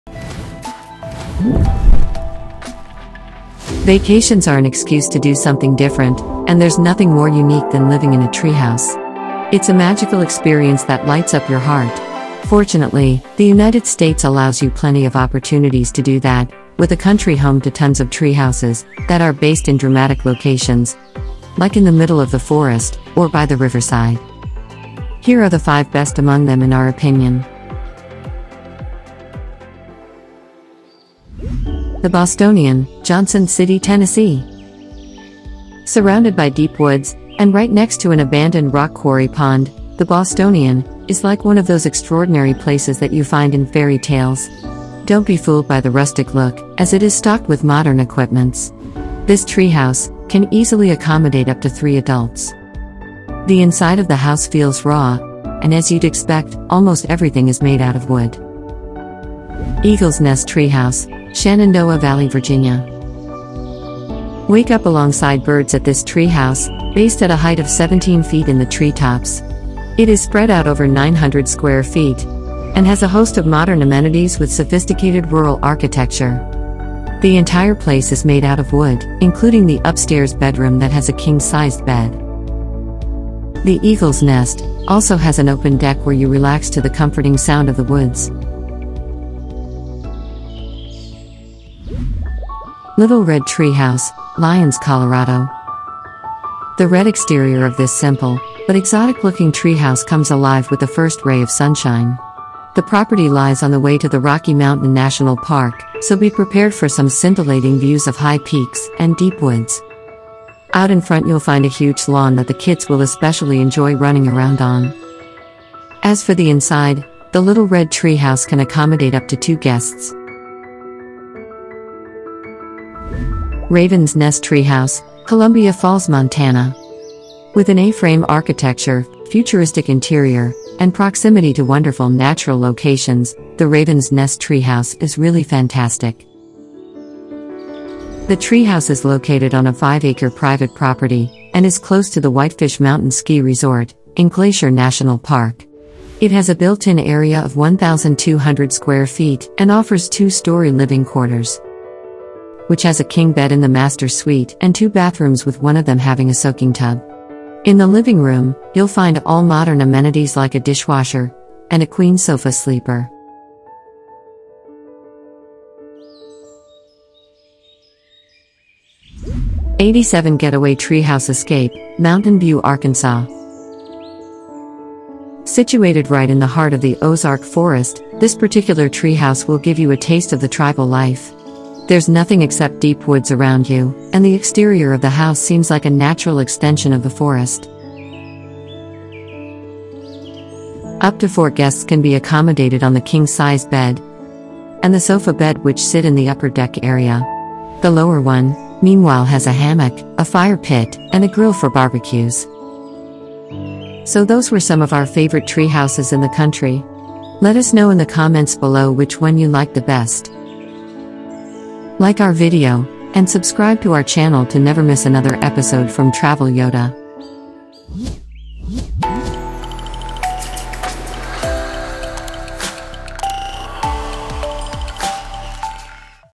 Unique Treehouses to Rent for Your Next Vacation
Vacations are an excuse to do something different, and there’s nothing more unique than living in a treehouse. It’s a magical experience that lights up your heart and makes you wanna do it again and again. Fortunately, the United States allows you plenty of opportunities to do that, with the country home to tons of treehouses that are based in dramatic locations, like in the middle of the forest or by the riverside. Here are the 5 best among them in our opinion.